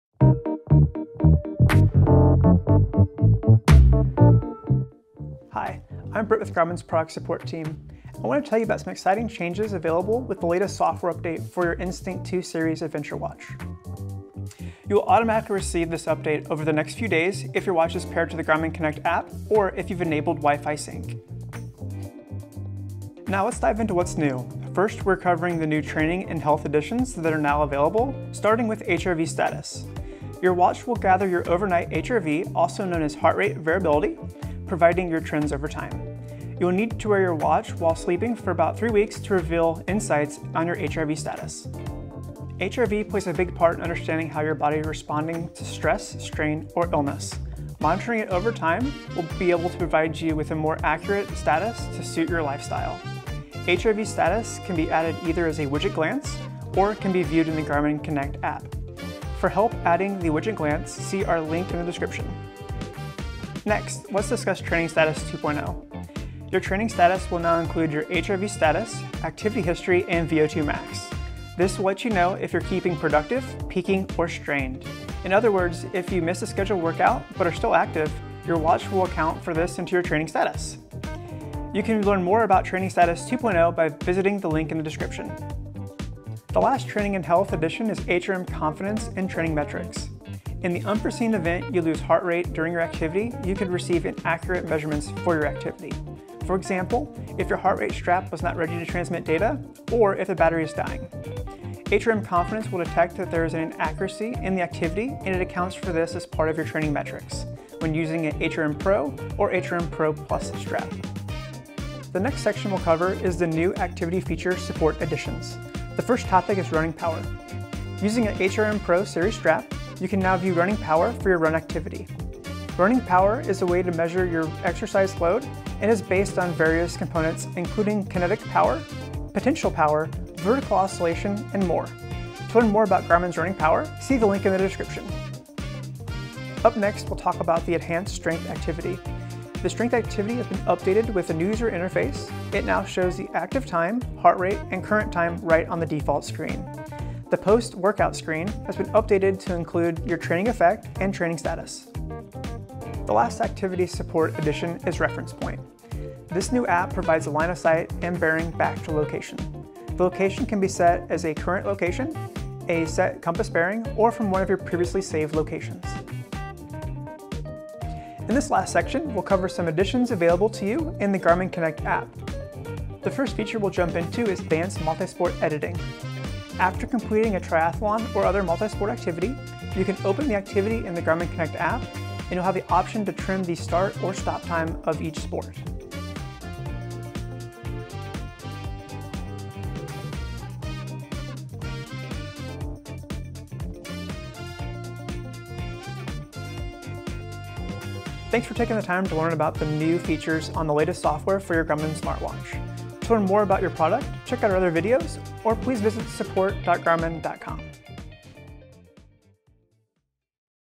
Support: Q3 2022 Software Update | Instinct® 2 Series
Learn about some of the new features coming to your Instinct® 2 series smartwatch with the 3rd quarter (2022) software update.

0:00 - Introduction
0:22 - Health & Training
1:51 - New Activity Support Additions
2:47 - Added Garmin Connect™ Application Features